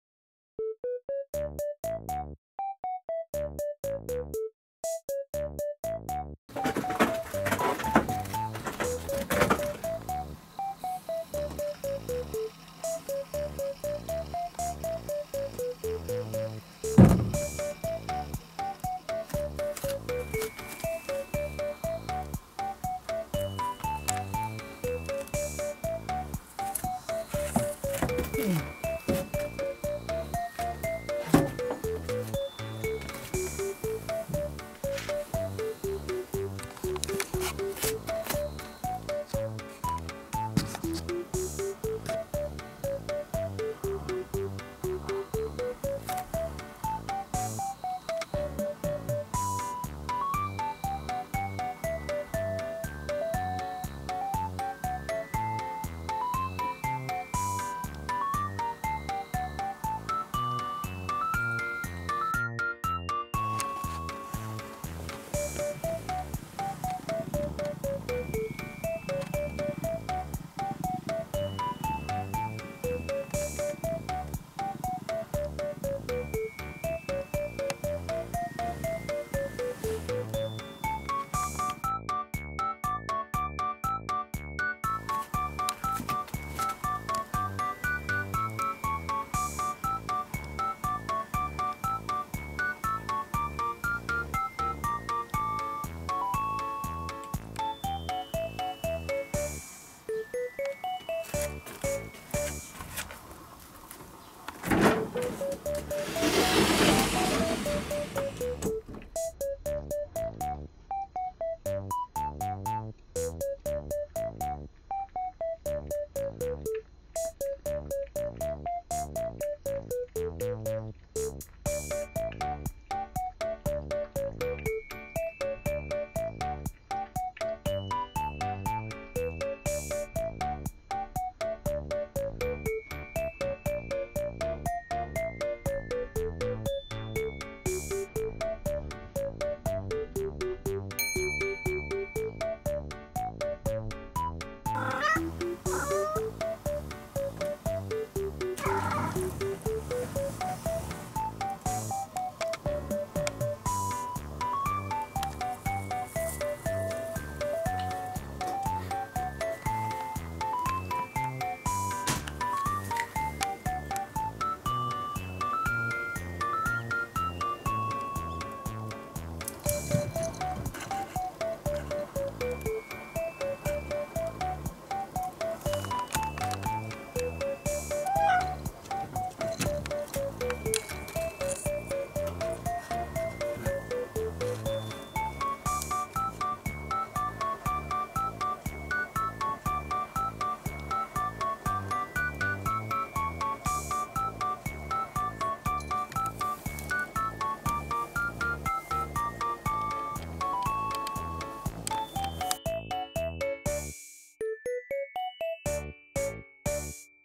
朝に戸を開けると外で待っていた野良猫キジトラさん
進展してるけど驚くほどゆっくり・・